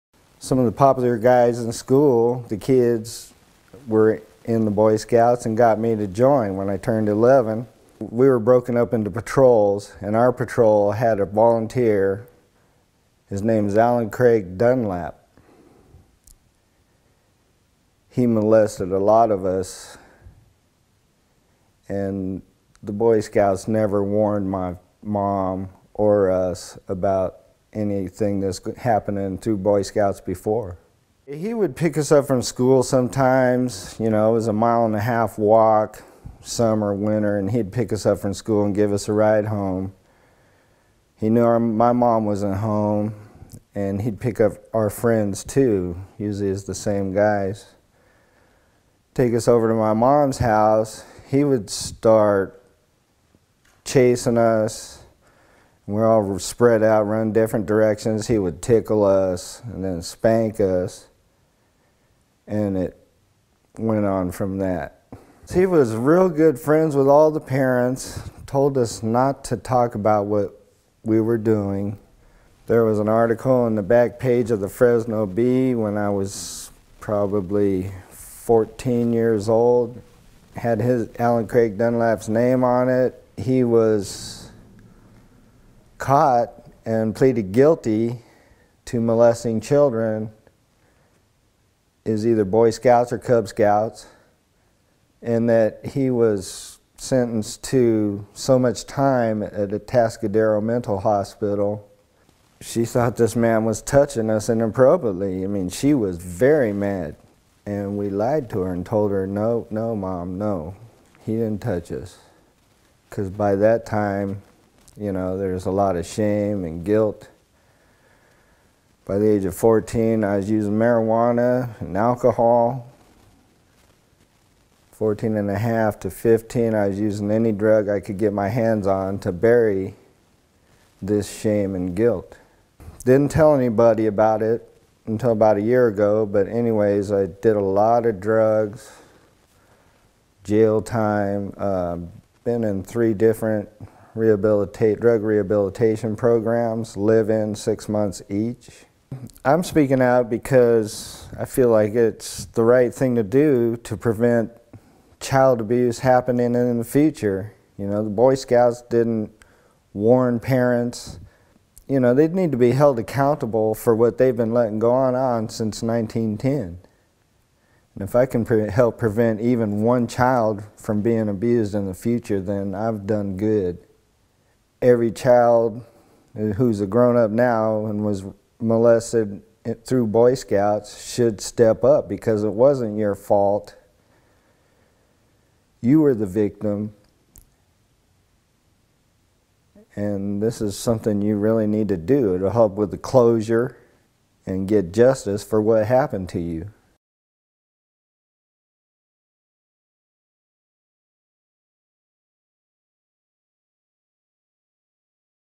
Fresno man comes forward about being molested by a Boy Scouts leader in the 1970s
After decades of hiding his painful past, David Green of Fresno decided to come forward about attacks by convicted child molester Alan Craig Dunlap so that he might help others in the future.

Video by: Craig Kohlruss